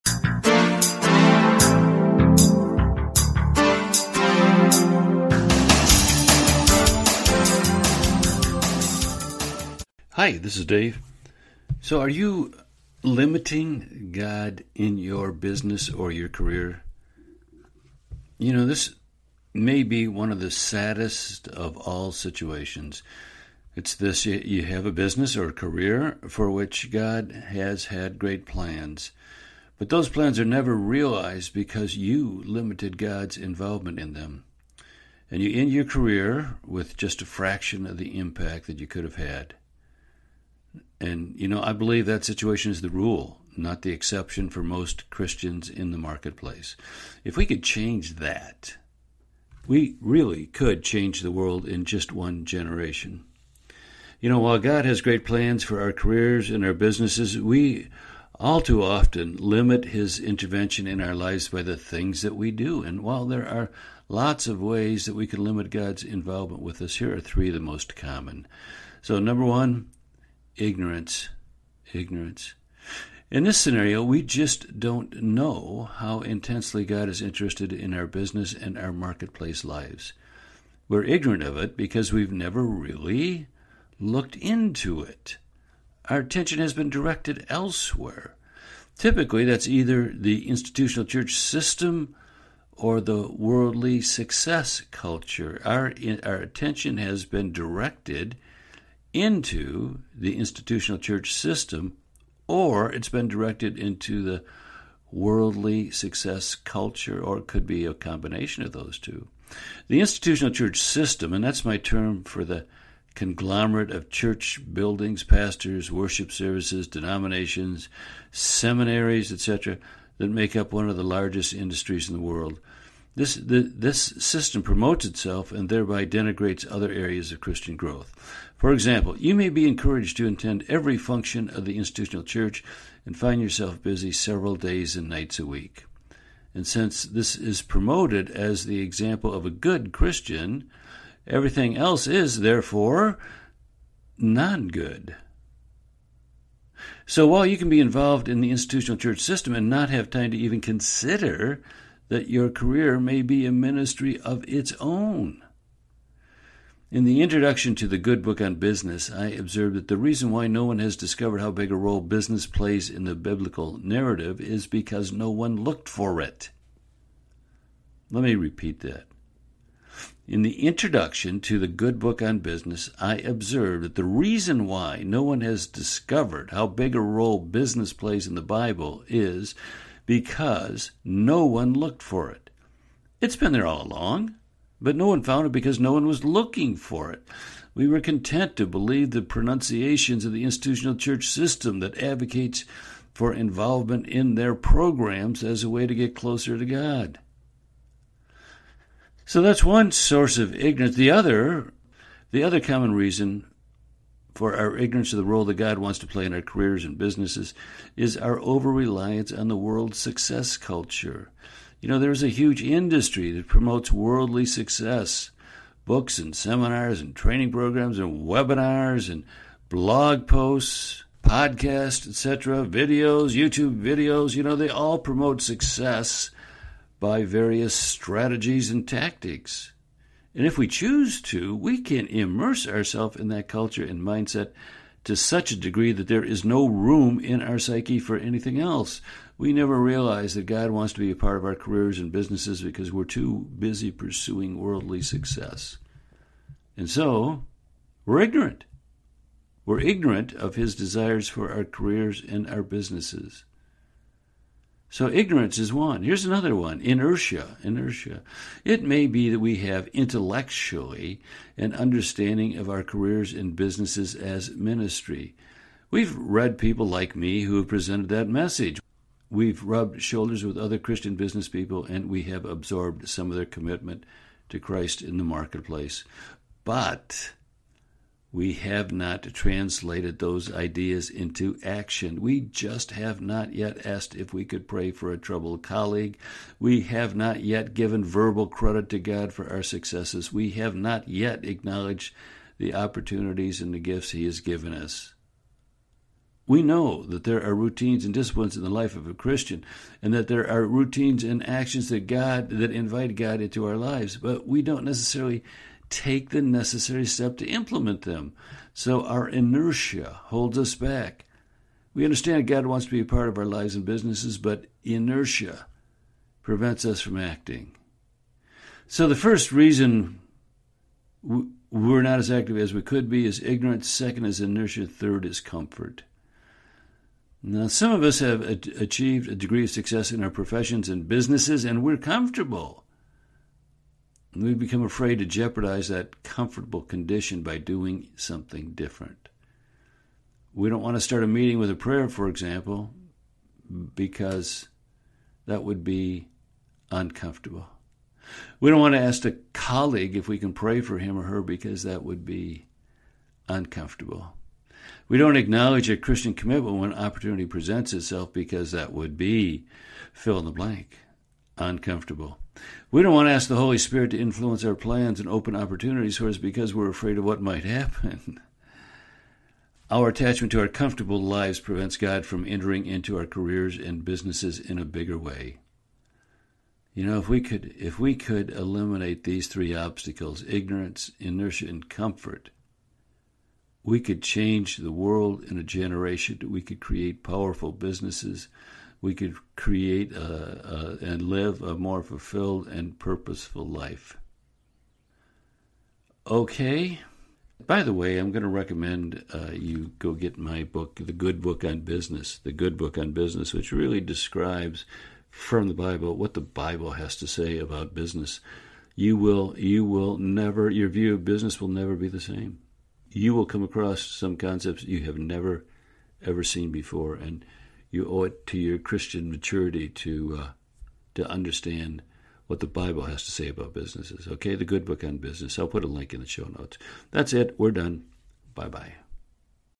Are You Limiting God In Your Business/Career?
This must be one of the saddest of all situations; You have a business or a career that God has great plans for, but they are never realized because you limited God’s involvement in them.  You end your career with just a fraction of the impact you could have had. Let’s dig into preventing this.